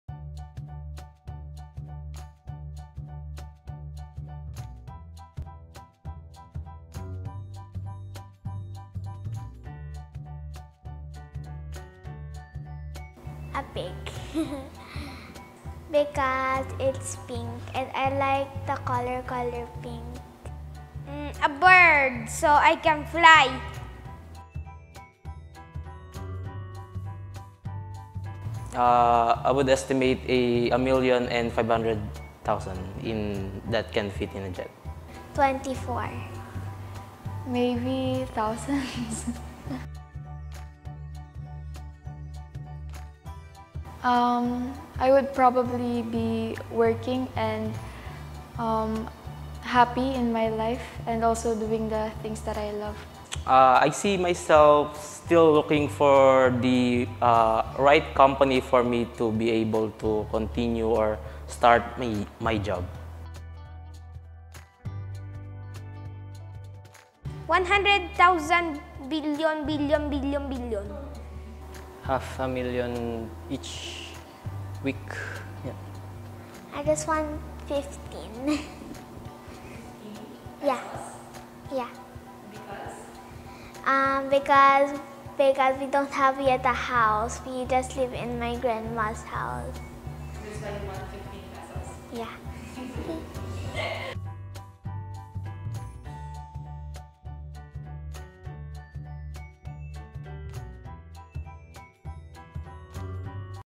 Kids Answer Job Interview Questions
We tried asking our students job interview questions and this is their response!